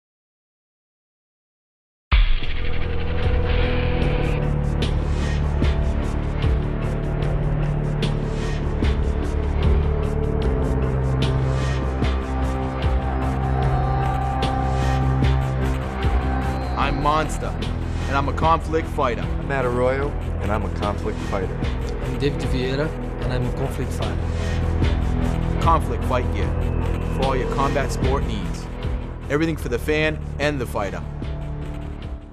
Conflict Commercial
Commercial featuring Monstah Lobstah, Matt Arroyo and David Vieira.